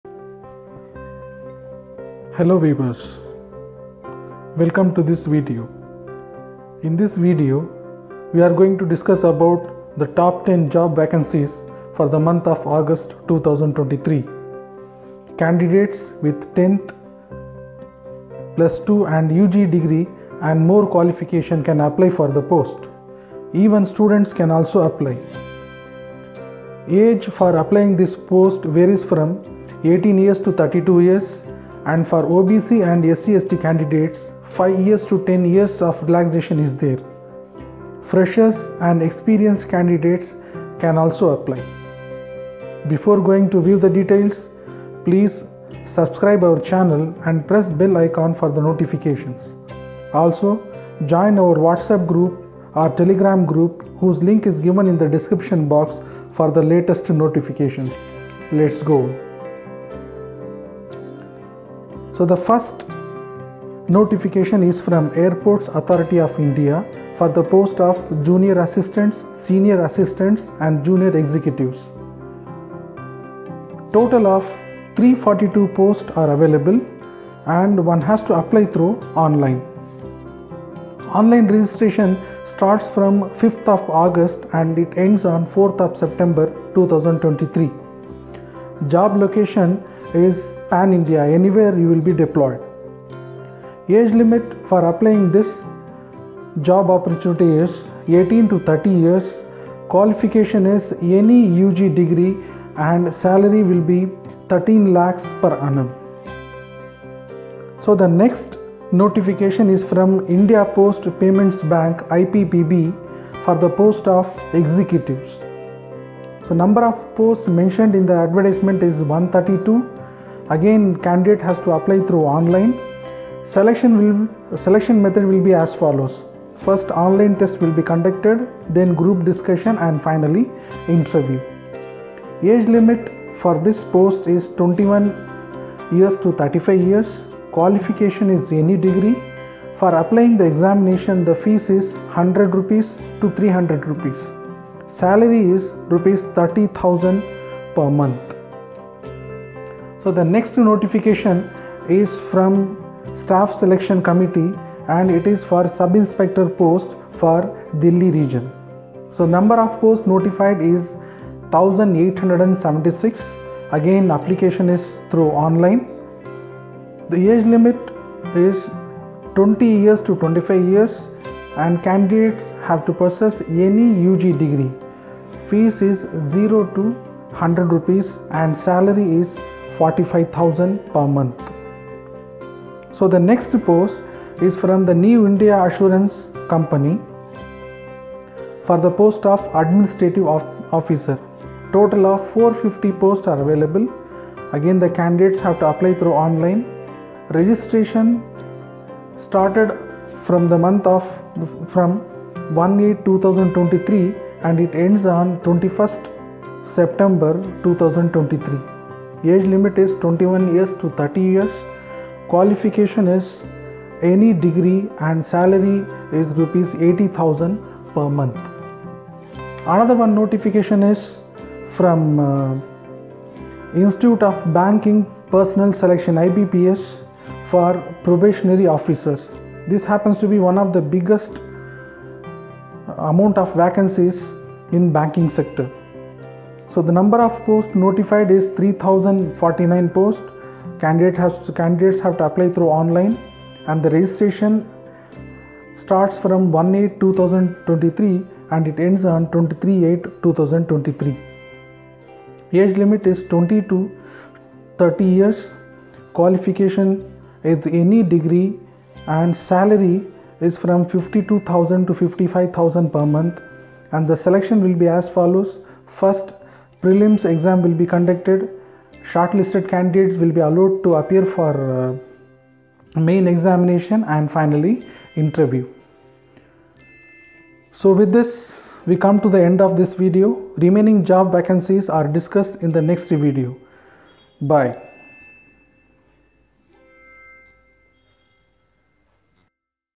Top Government Job Opportunities in August 2023-Part1 | Government Permanent Jobs with Good Salary
In this Video, we are going to discuss about the various government job opportunities for the month of August, 2023

The vacancies are from
Airports Authority of India Recruitment,
IPPB Recruitment,
Staff Selection Committee,
The New india Assurance,
IBPS